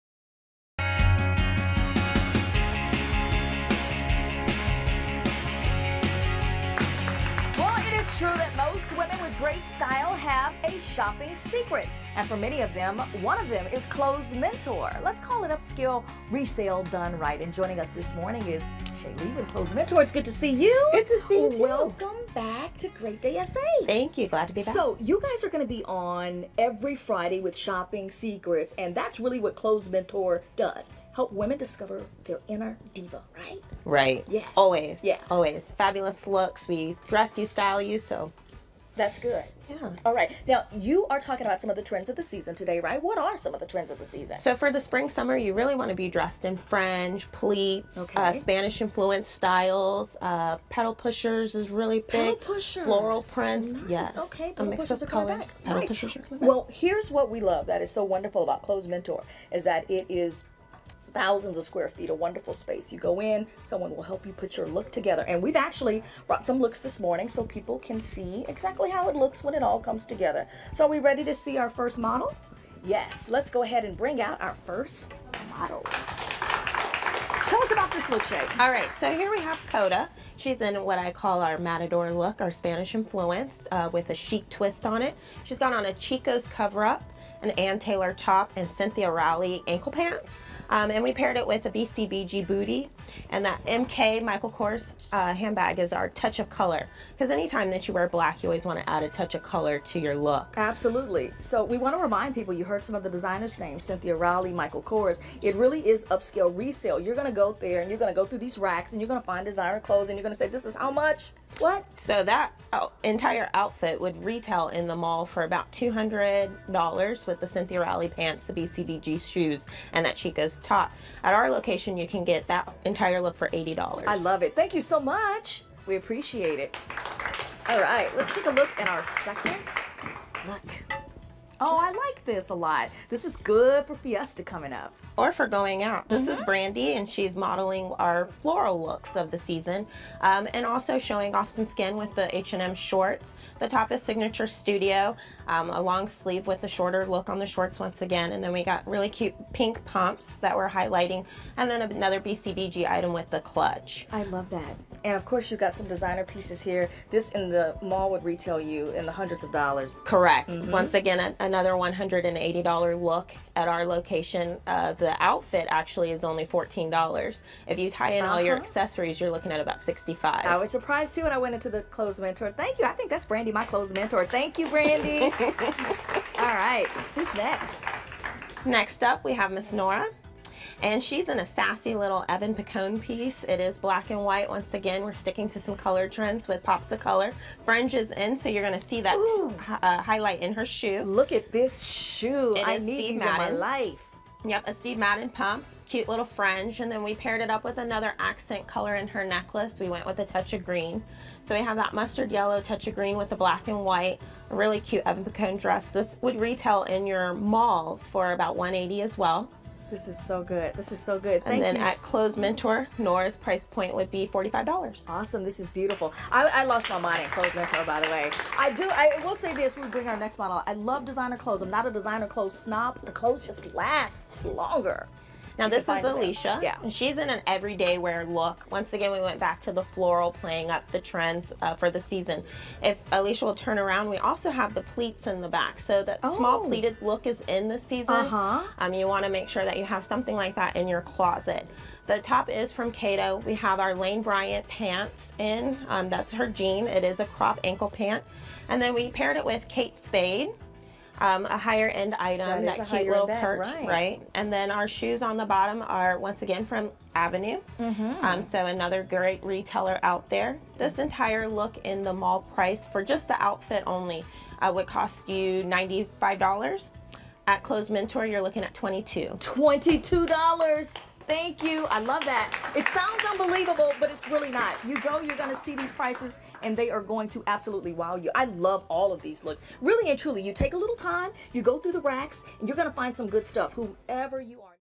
Spring 2016 Trends from Clothes Mentor San Antonio
Spanish influence, floral patterns and more are big this spring. Find out what else is trending in this TV spot featuring Clothes Mentor in San Antonio. You’ll always find high-end, fashionable looks for great prices at your local Clothes Mentor.